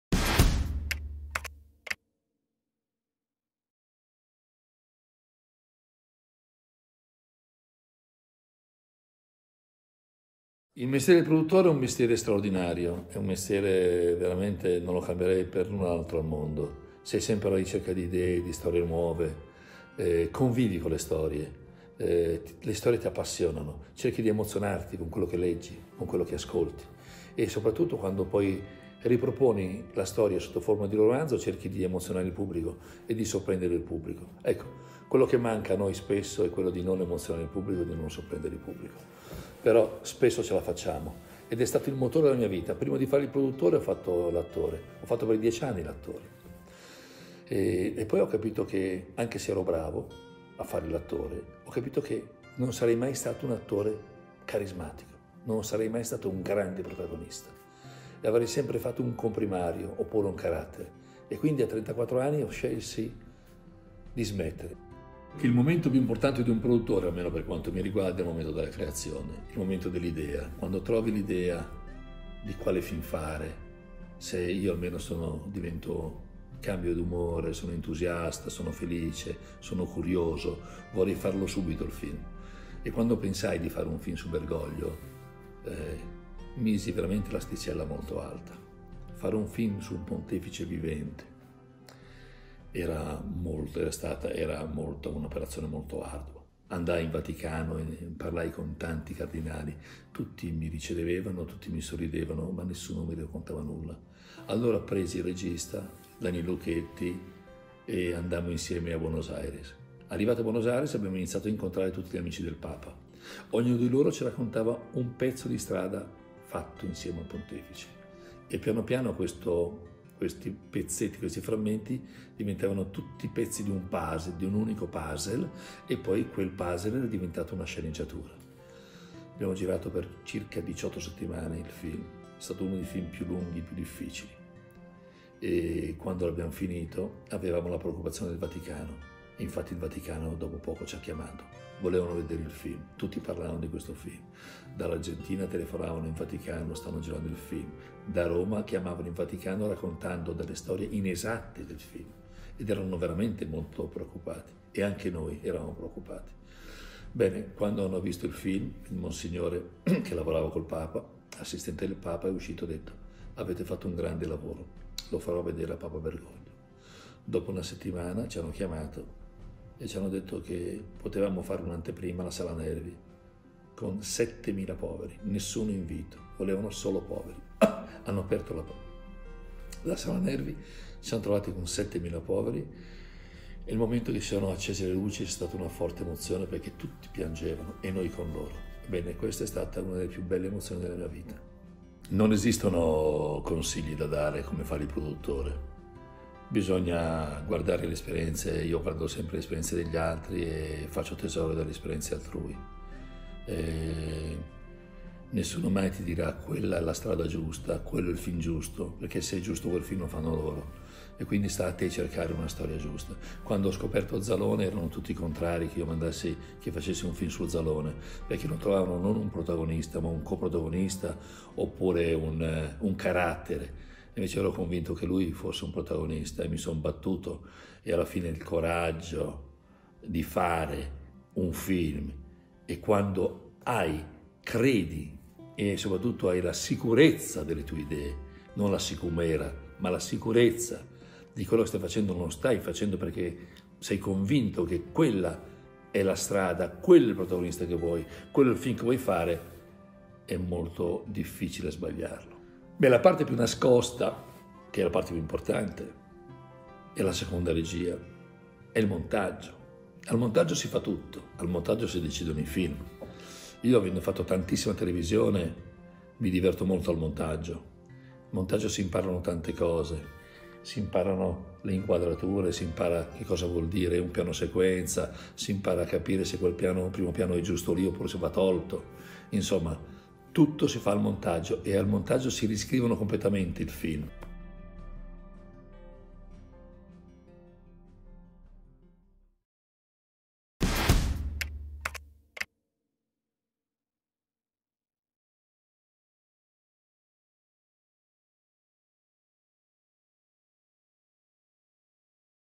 Fare Cinema: REBOOT. ITALIAN CINEMA RESTARTS | Pietro Valsecchi, producer
Pietro Valsecchi, with Camilla Nesbitt, has invented the “Zalone phenomenon”: Valsecchi is one of the major cinema and TV series producers in Italy. In this video he talks and explains about his profession and its lesser known aspects.